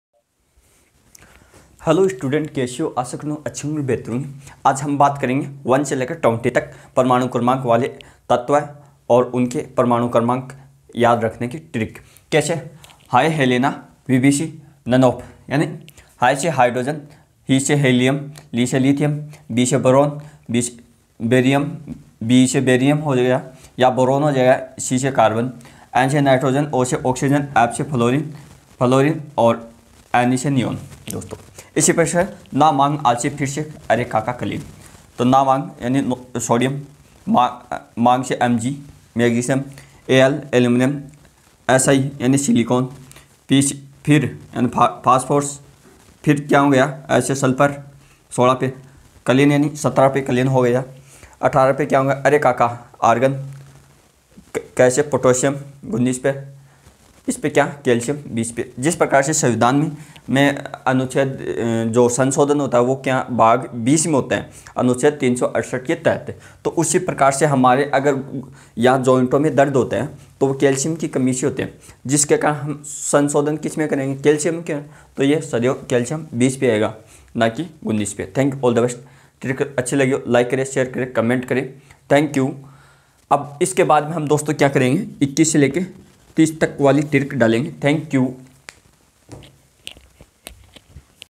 1से 20तक परमाणु क्रमांक वाले तत्त्व को याद करने की ट्रिकBY:-k2sir motivational speaker
periodic table